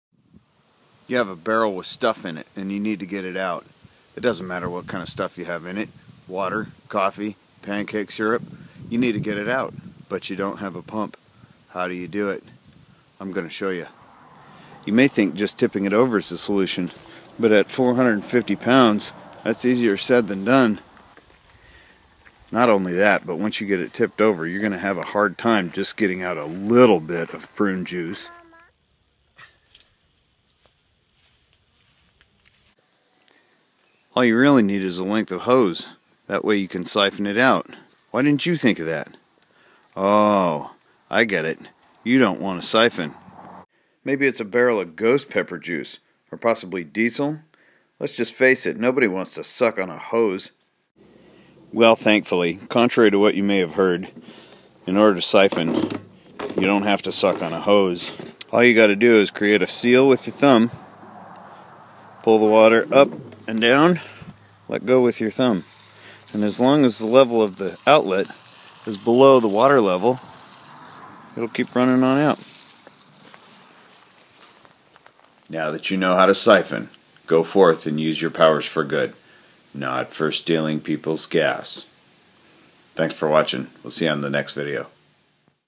How to siphon (without sucking a hose)
Need to siphon something but don't want to suck on a hose?
Here's how